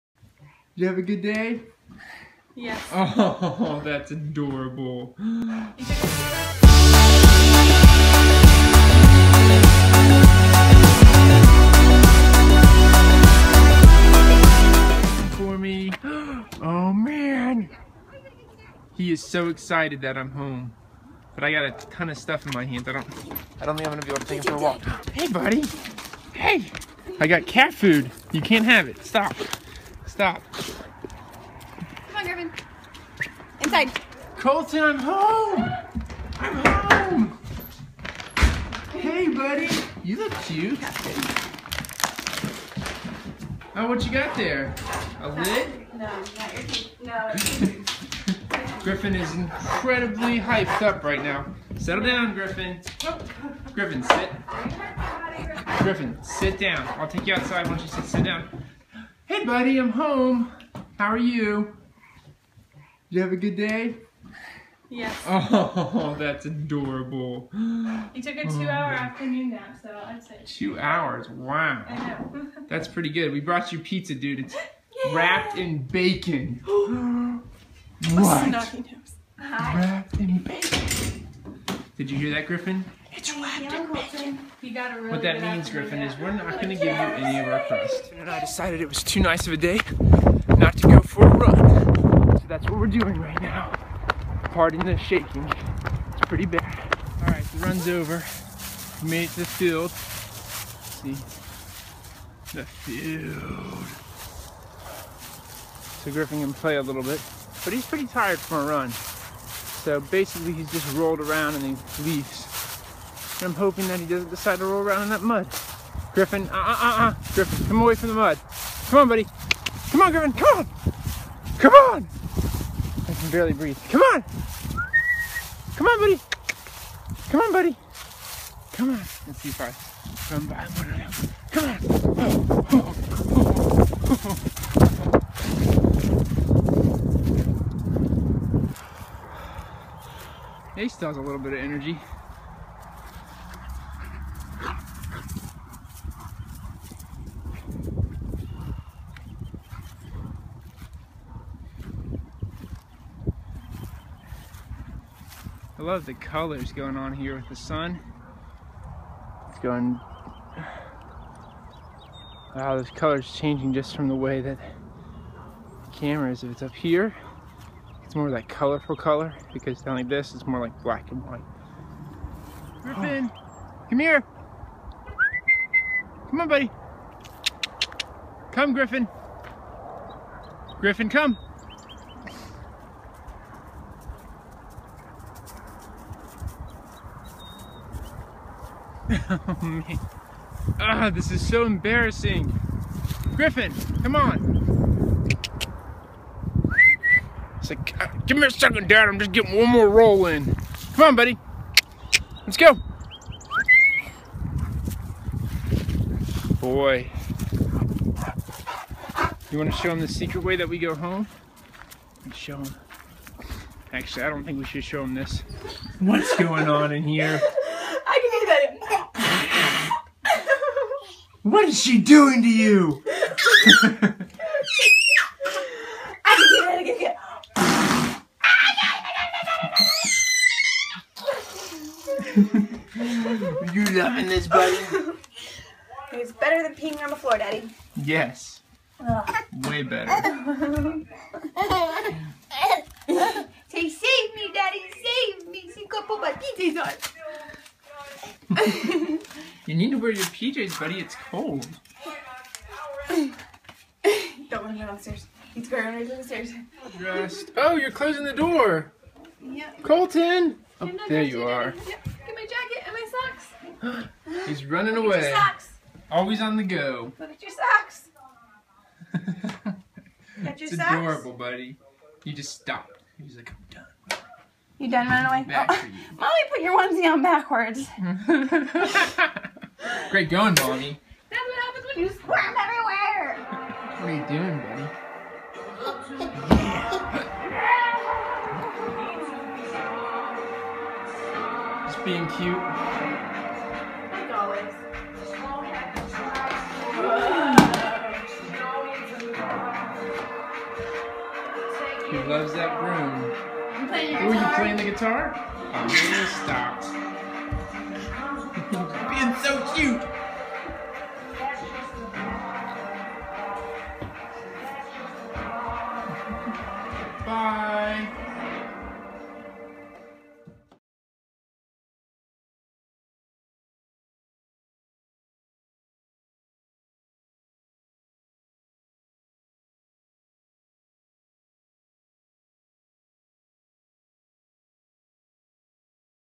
A TODDLER AND HIS GREAT DANE MEET DAD AT THE DOOR
Griffin gets a much needed run.